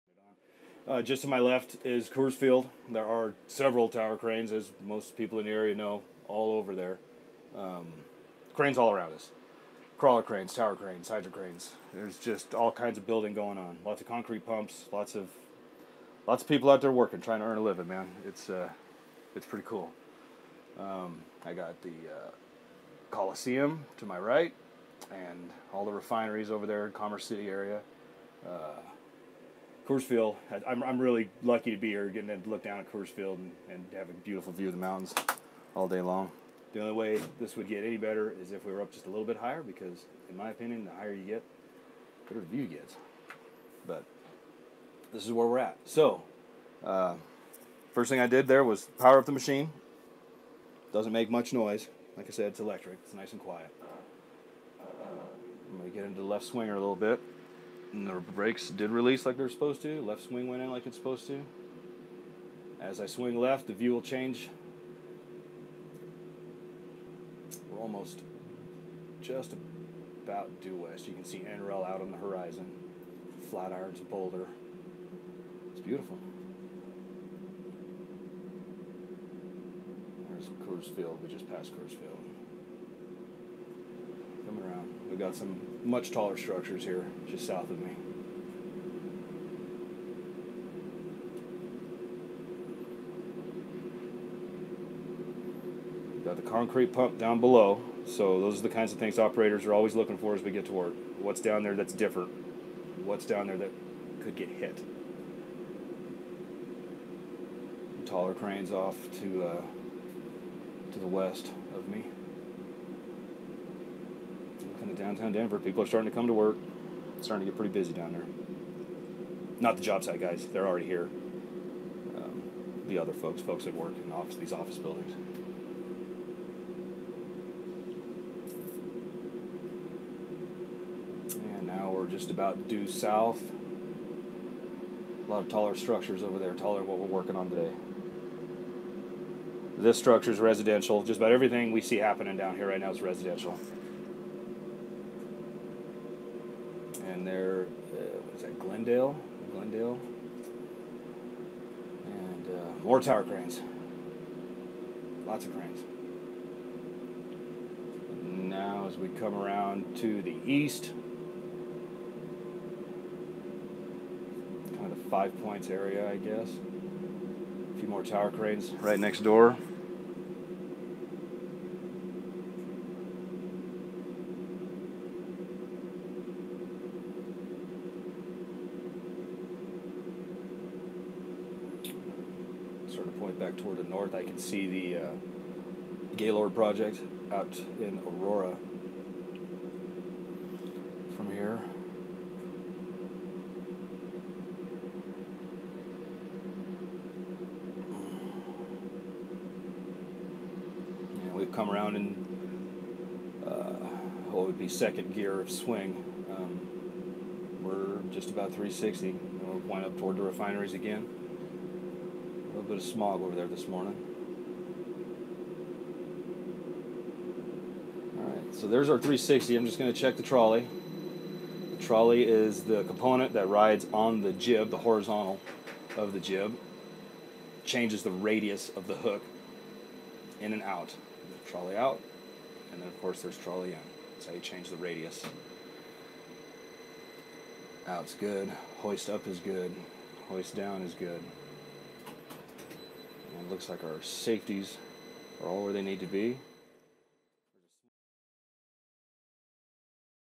Denver Downtown Tower Crane 360°
Tower Crane Operator Aaron Genova conducts a 360° safety check of his crane and give us a tour of the skyline of Downtown Denver.